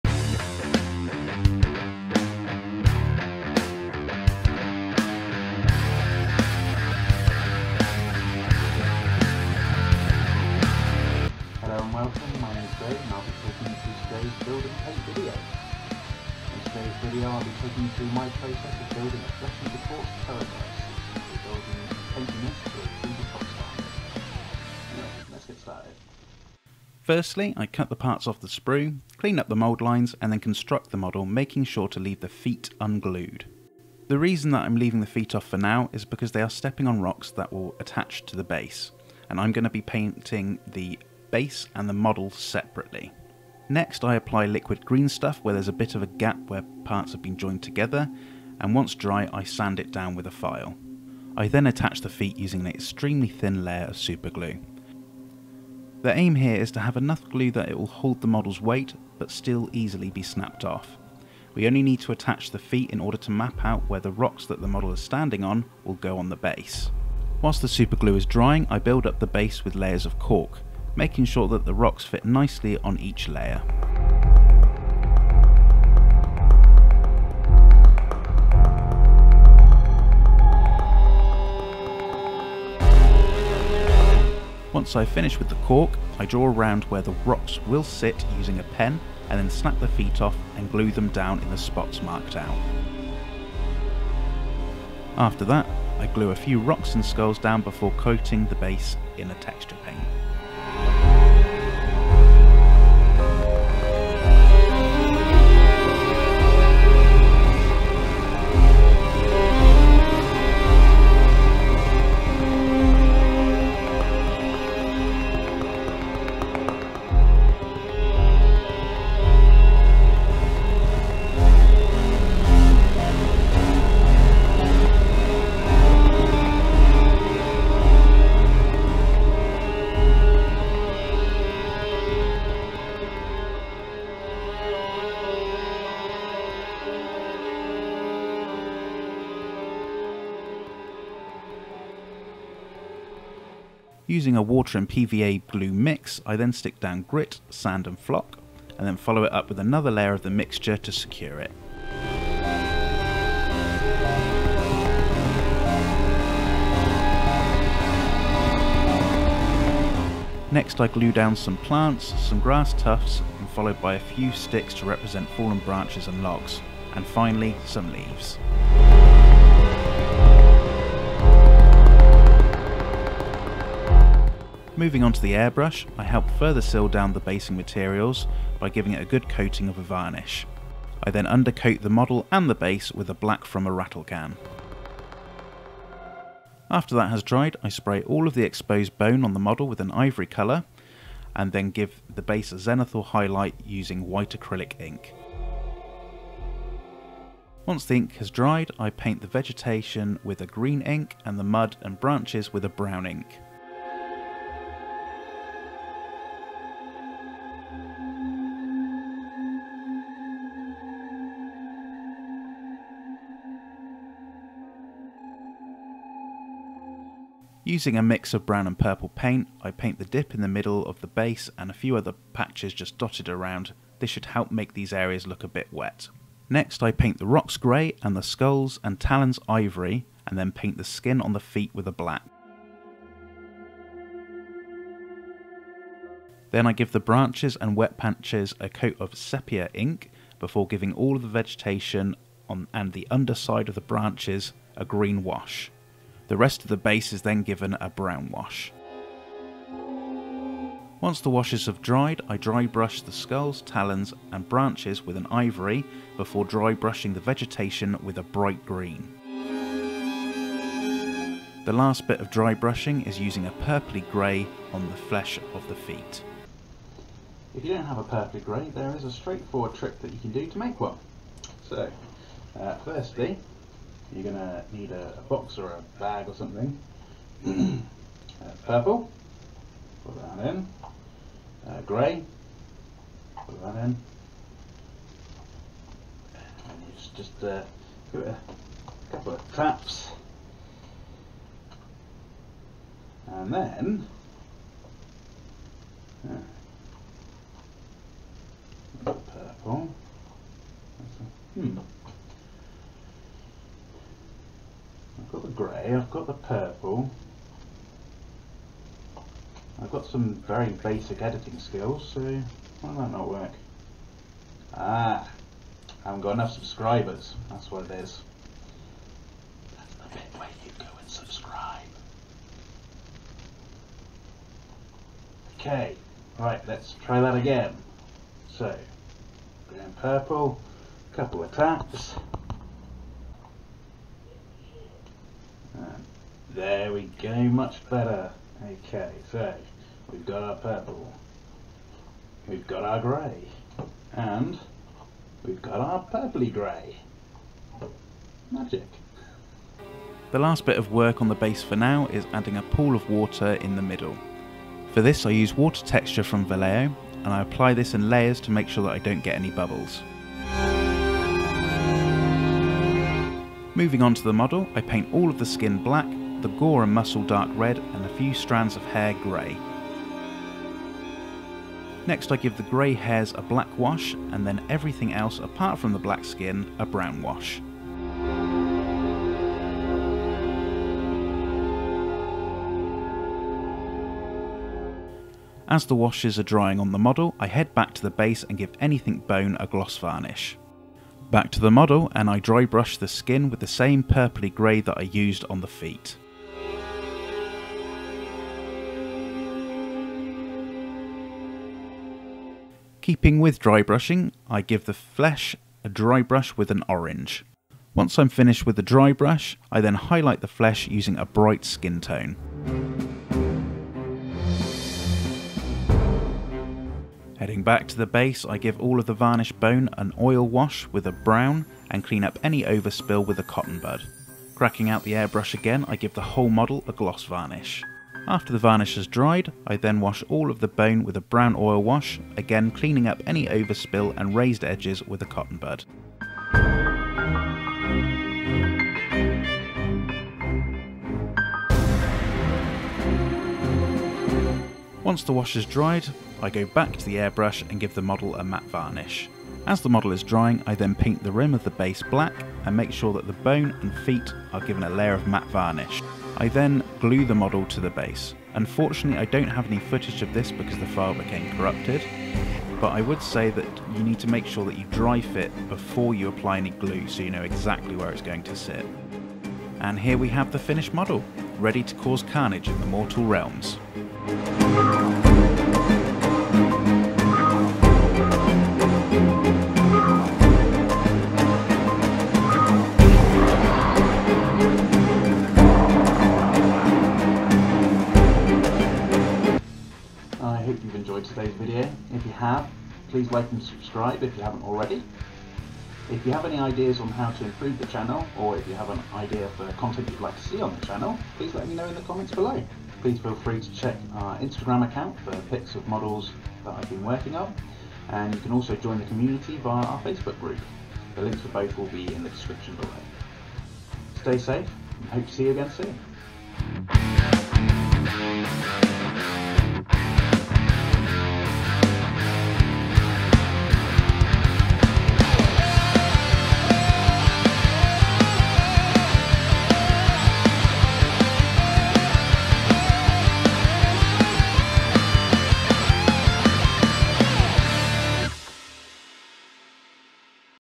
How to Build and Paint a Terrorgheist to a Table Top Standard
Join me as I go through my process for building and painting a Flesh Eater Courts Terrorgheist from Warhammer Age of Sigmar. This will be done to a table top standard with a jungle themed base.